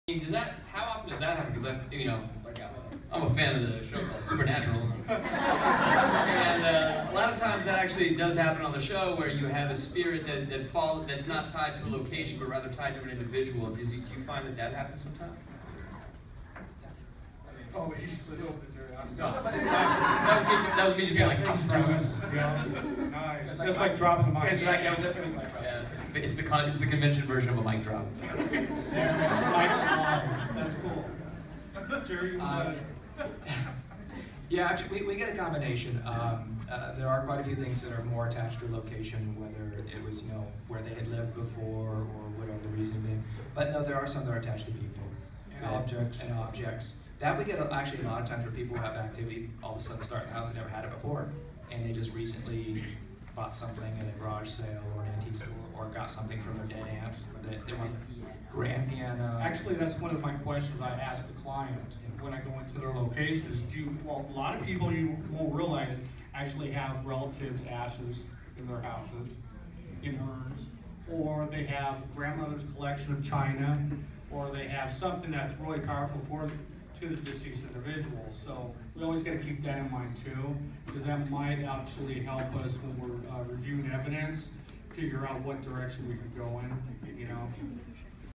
Travis Wester (from Supernatural) asks about Haunted People rather than Haunted Places
Travis Wester played the fan-favorite character Harry Spangler, one of the Ghostfacers, on the long-running hit TV show SUPERNATURAL. Here, he discusses real-life haunted objects with at the Long Beach Comic Con with members of TAPS West Coast, the real Ghosthunters from Syfy's hit TV series.